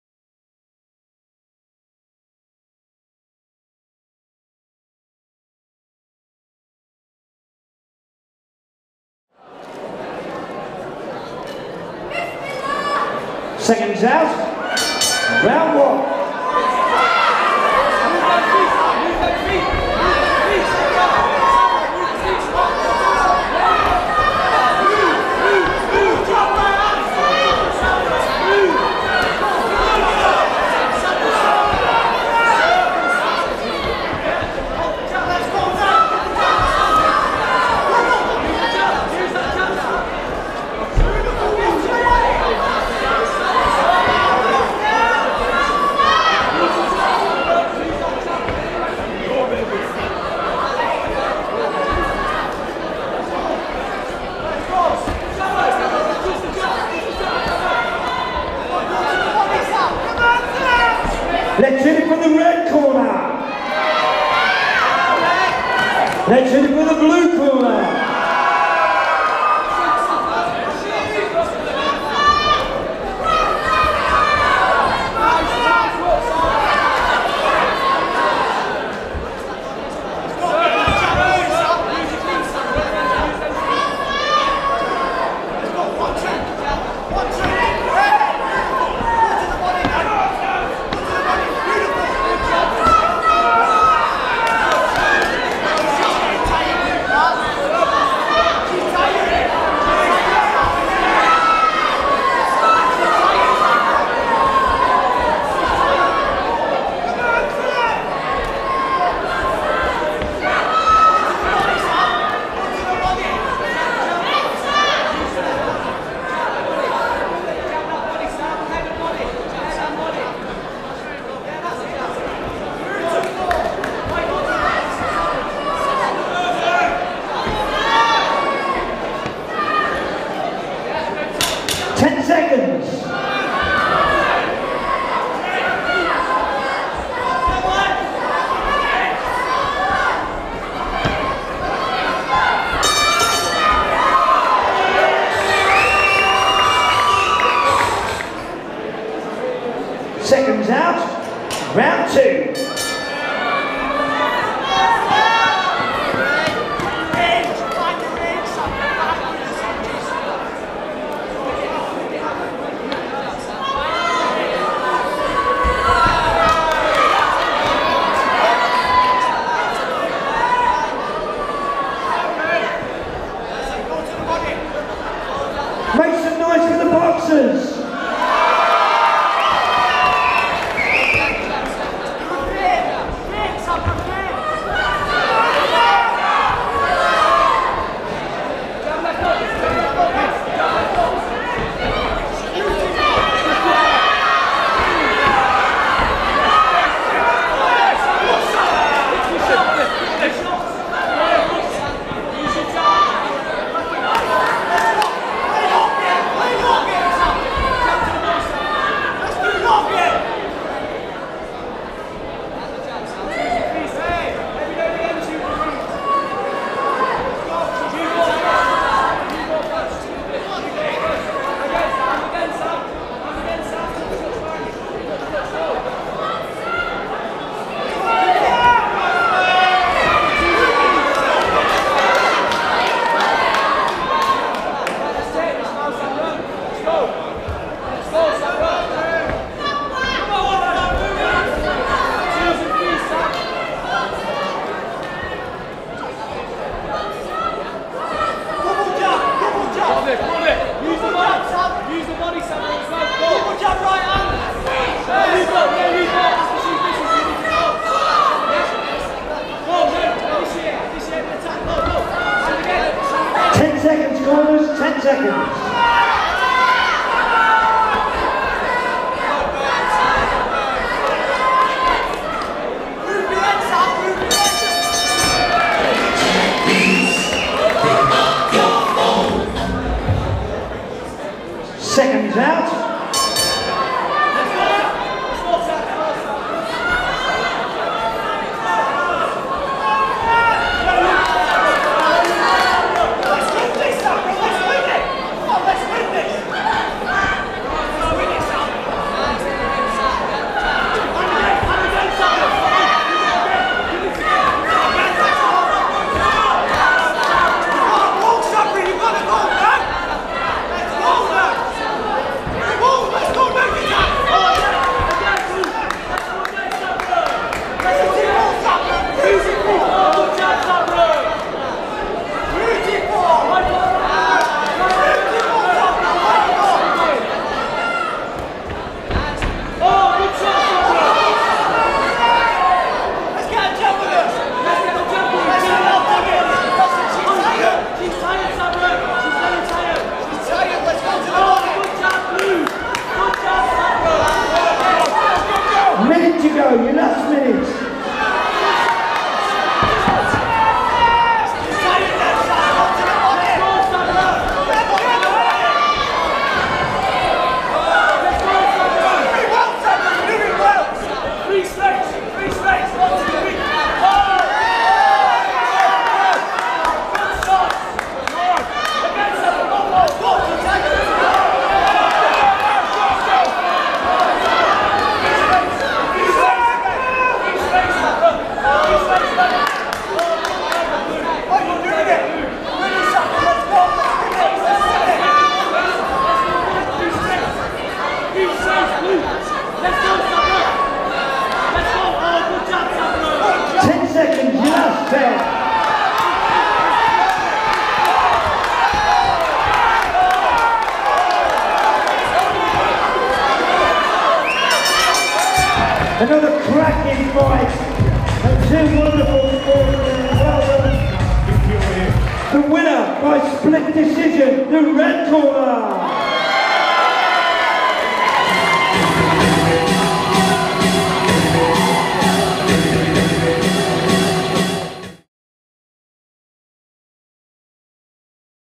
Ultra White Collar Boxing | Bolton | Sam Platt VS Sabra Latiff
Bolton Ultra White Collar Boxing 8th July 2017

8 Weeks FREE training / Get in the best shape of your life/ Raise money for Cancer Research UK